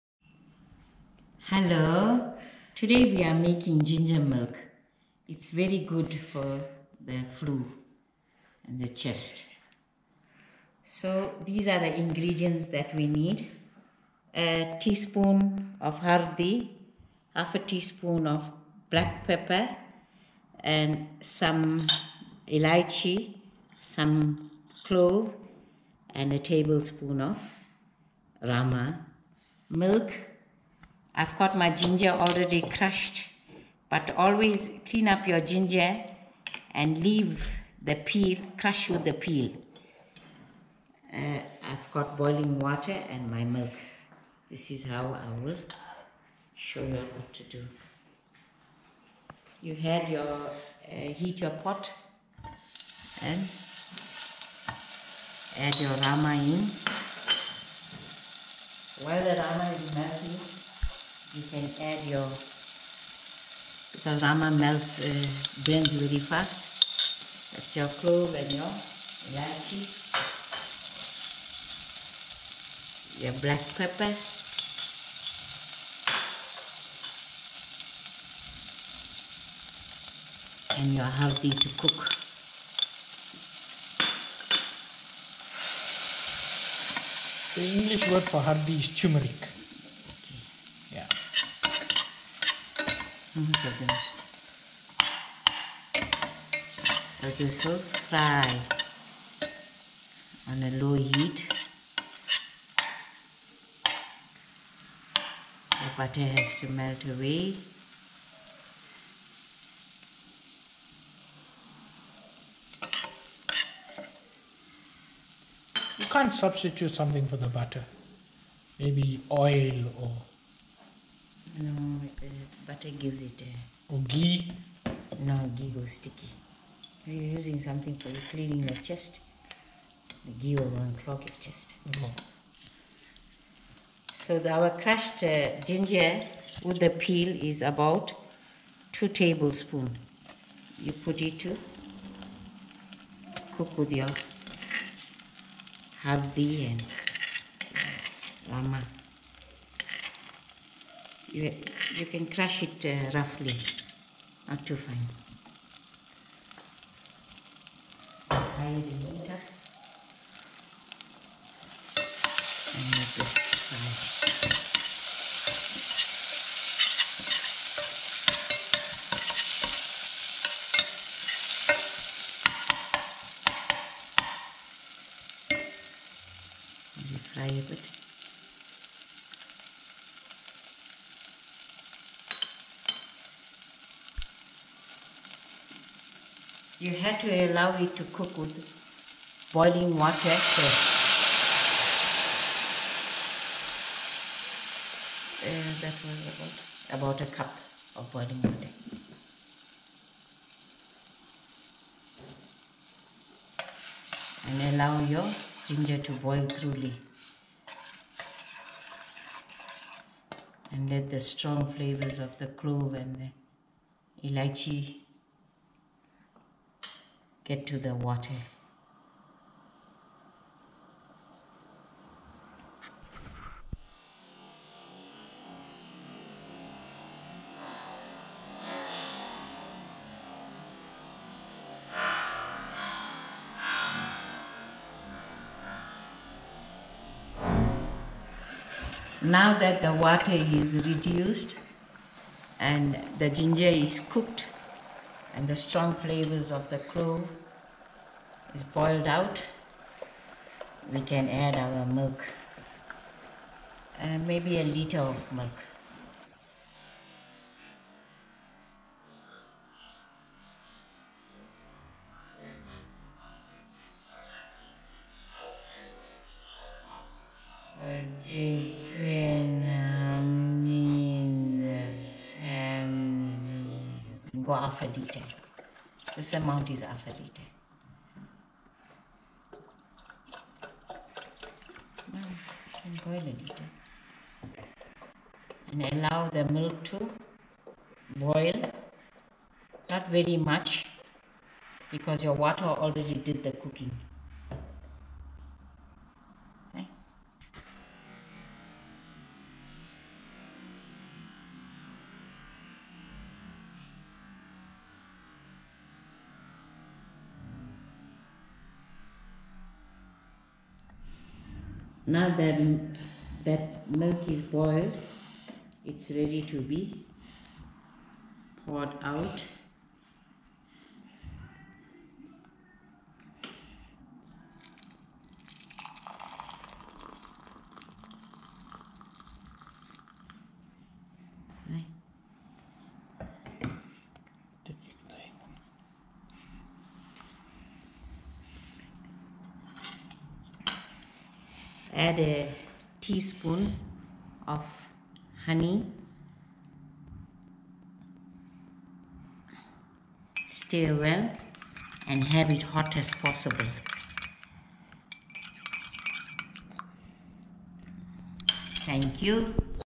Making Home Made Gingermilk
Now known more as a Tumeric latte in some circles, this is a recipe for ginger milk, to be drunk as a home remedy for tight chests and coughing!